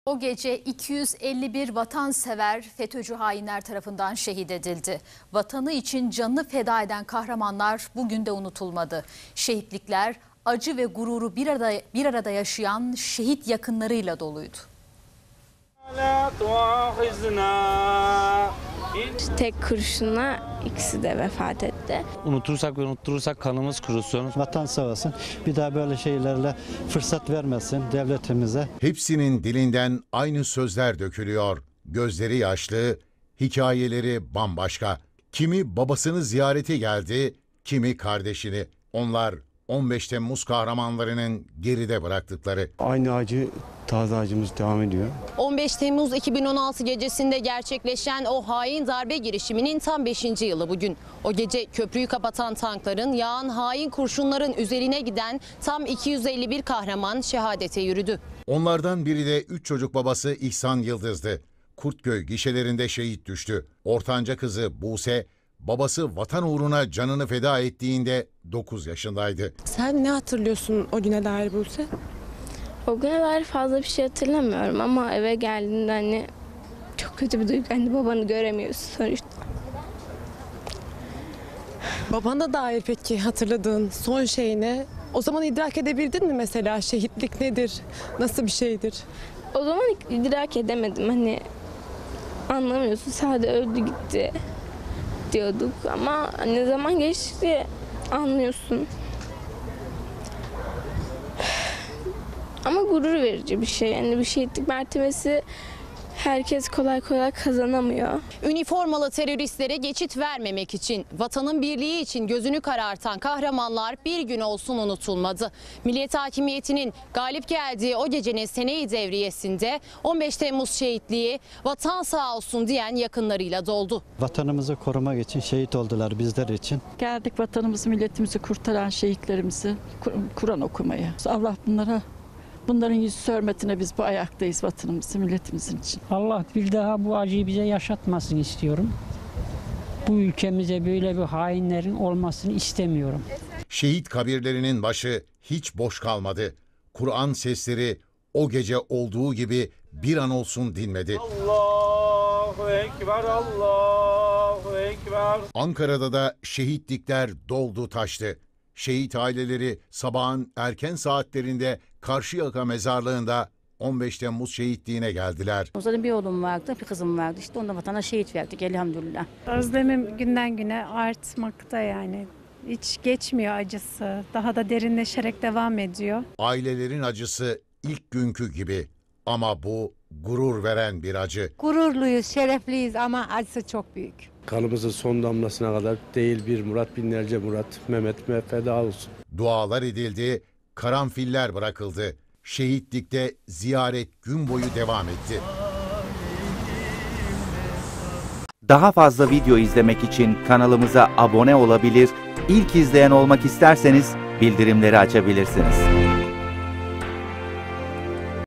15 Temmuz'un Kahramanları Vatan Uğruna Can Verdi
Vatanı için canını feda eden kahramanlar unutulmadı. Şehitlikler acı ve gururu bir arada yaşayan şehit yakınlarıyla doldu.